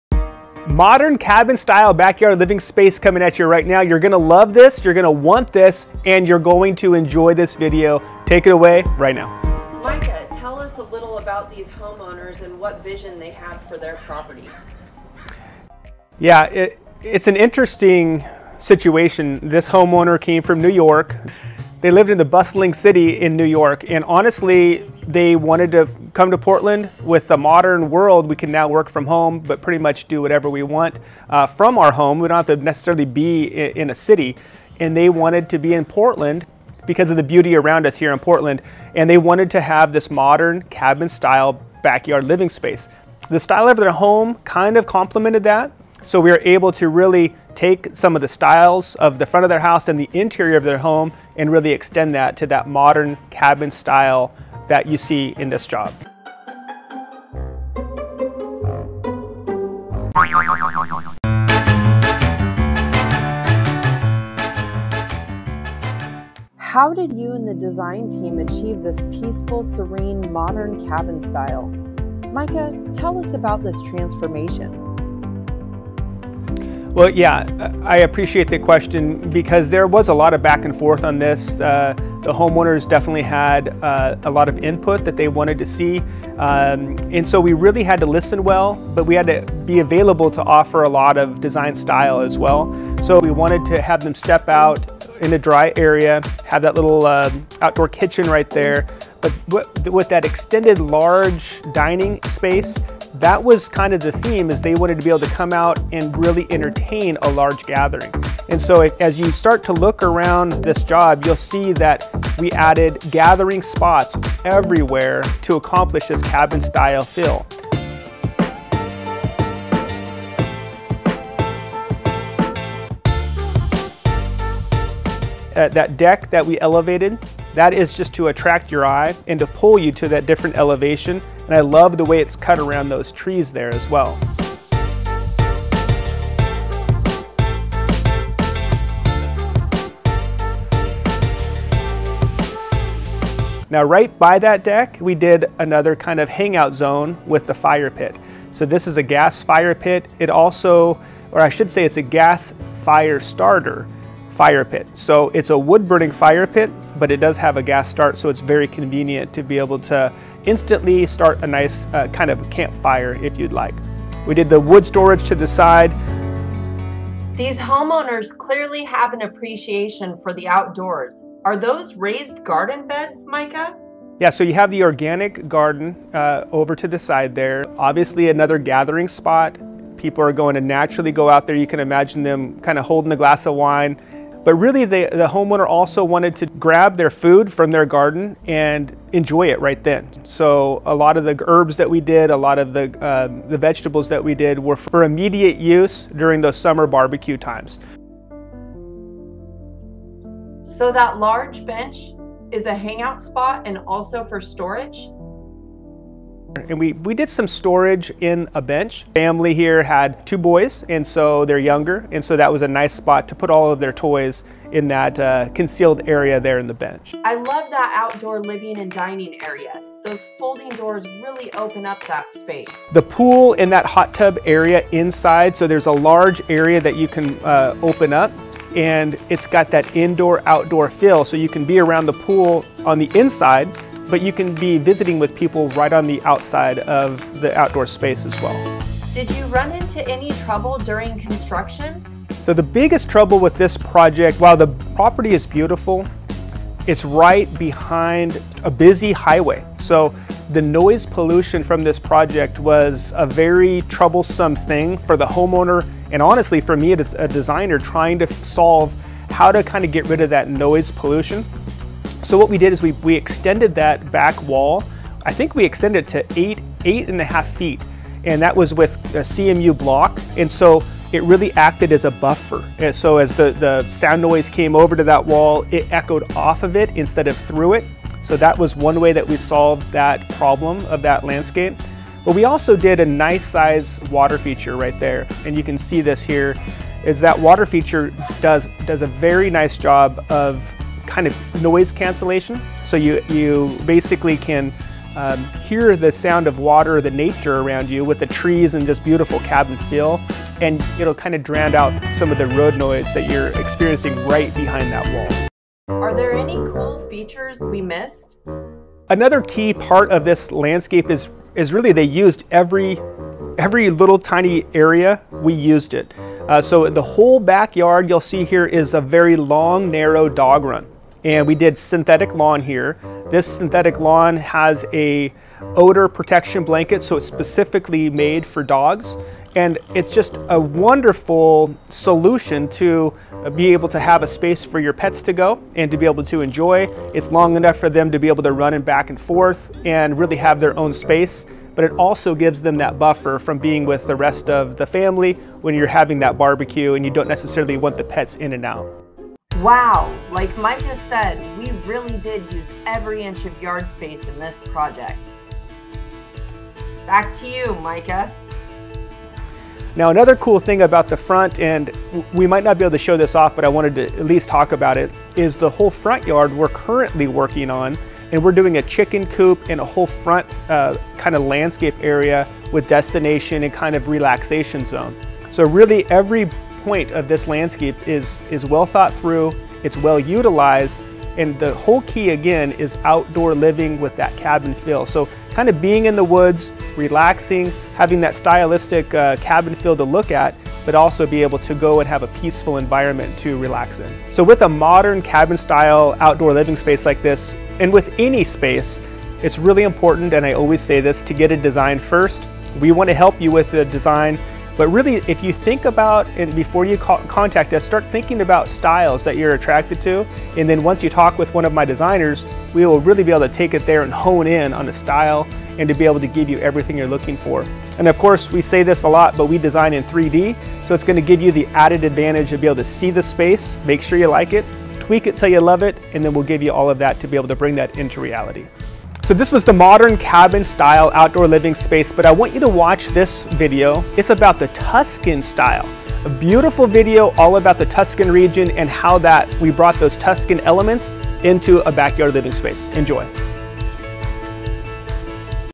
Modern Cabin Style Backyard Design
A very popular home style is the Modern Cabin Style home. This project shows how to bring the Modern Cabin Style into the Backyard Living Space.